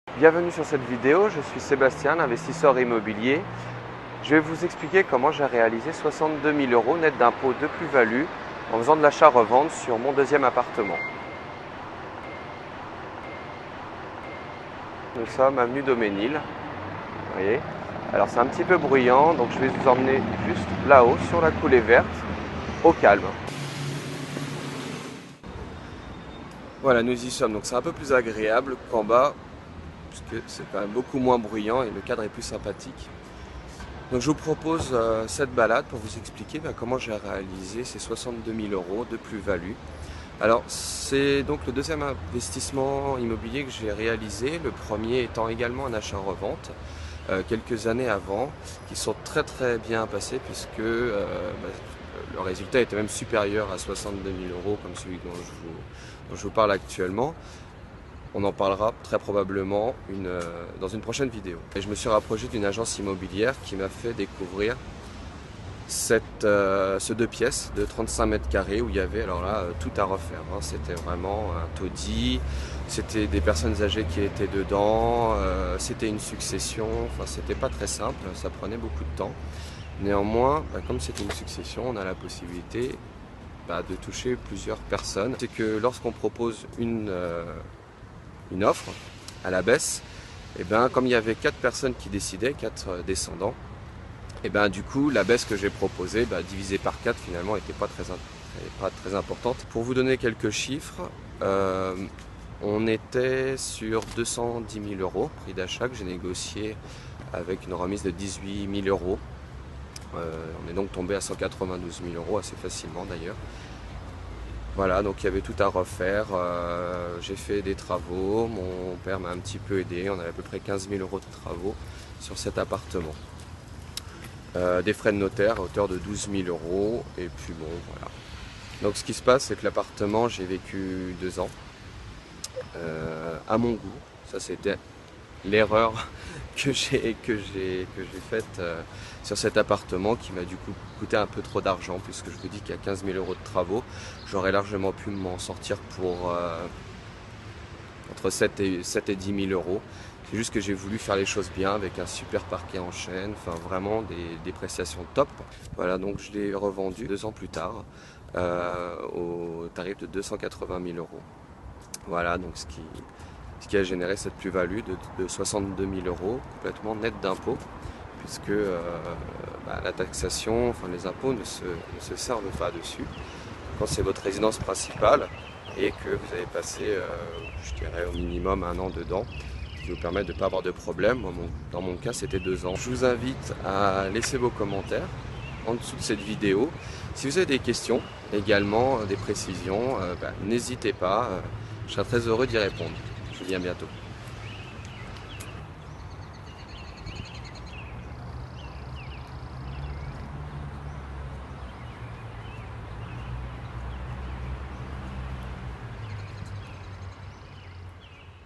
Immobilier : 62.000€ Nets d'Impôts (Sébastien D)
←Cliquez-ici pour voir l'article en entier !
Je vais vous expliquer dans cette vidéo comment j'ai trouvé mon 2ème appartement, négocié le prix de (-18.000€), réalisé les travaux, aménagé l'intérieur... Et revendu pour créer 62.000€.
Sébastien l'investisseur immobilier